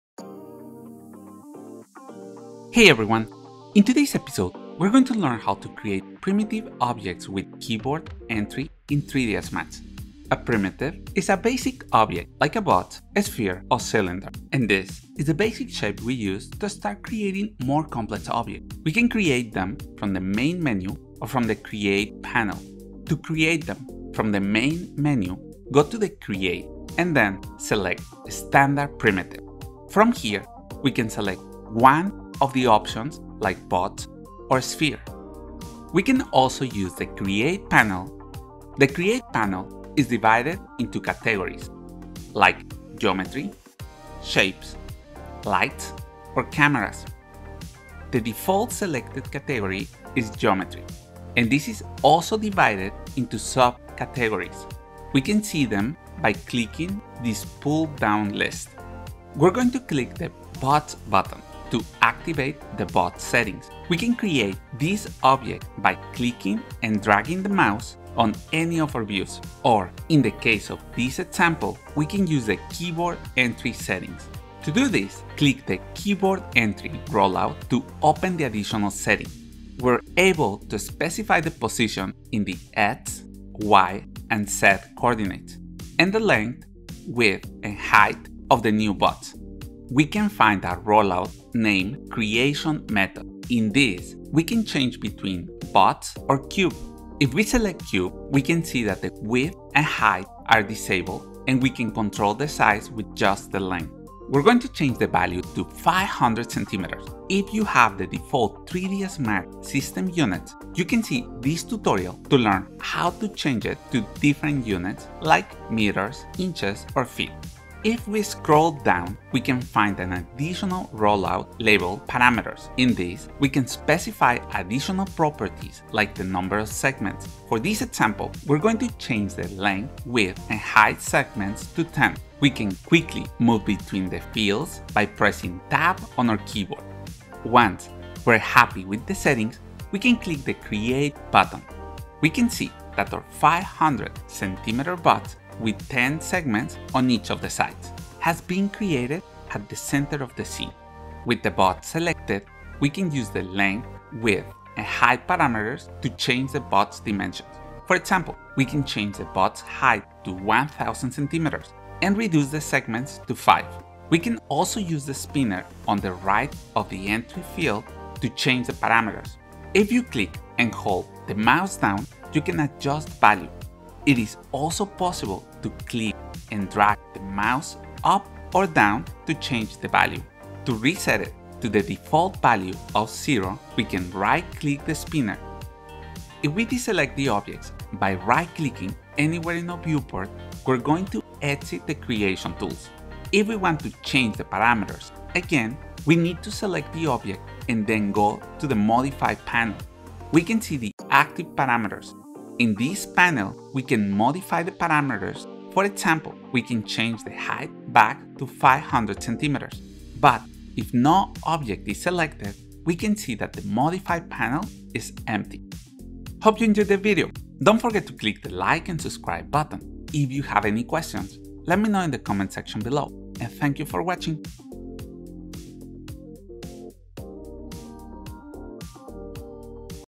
How to Create Primitive objects Using the keyboard entry in 3ds max
In this video, we are going to learn how to create primitive objects in 3dsmax using the keyboard entry. We'll go over the different commands and how to use them to create basic objects like cubes. We'll also see how to change the properties of the object we create. By the end of this video, you'll know how to create a primitive object in 3dsmax!

SKIP AHEAD ⏩
00:00 Intro
00:22 How to create standard primitives
00:58 Create a box
01:18 Keyboard entry options
01:29 Creation method options
01:44 Set the size of the cube with Lenght
01:57 Box Segments
02:17 Create button
02:31 Modify the box Parameters Options
03:36 Outro